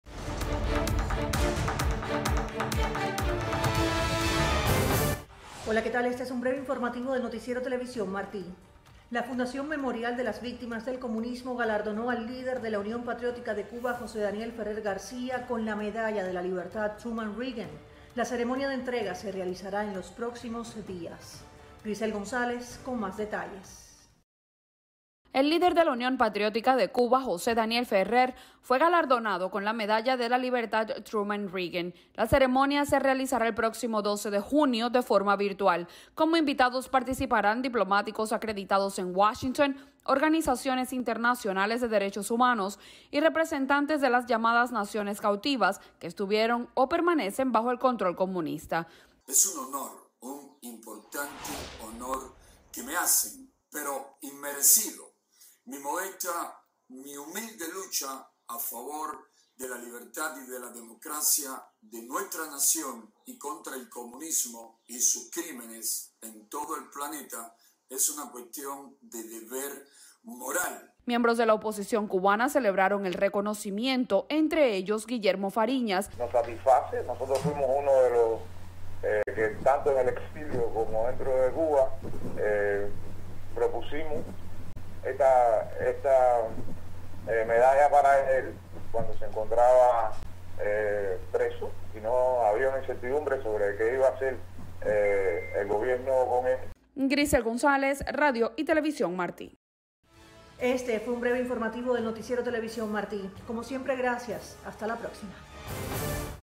Breve Informativo
La fundación memorial de las víctimas del comunismo galardonó al líder de la Unión Patriótica de Cuba, José Daniel Ferrer con la Medalla de la Libertad Truman-Reagan.